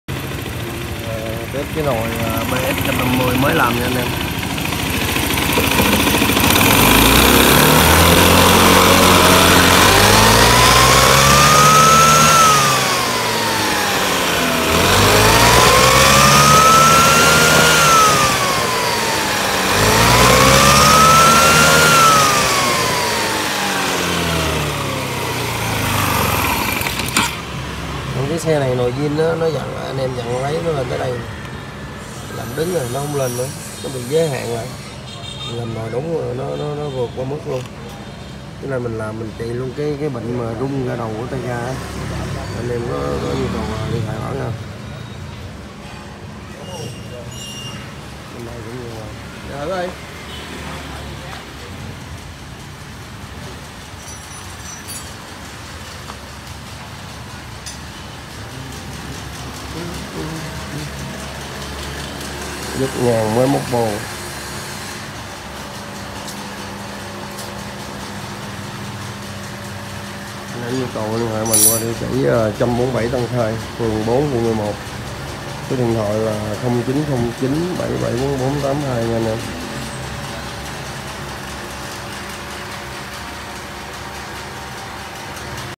Honda PS 150i làm nồi chạy nhẹ bốc tiết kiệm xăng hết rung ga đầu tại TBK Team
Honda PS 150i làm nồi chạy nhẹ bốc tiết kiệm xăng hết rung ga đầu tại TBK Team 147 Tân Khai P4 Q11 Bảo Hello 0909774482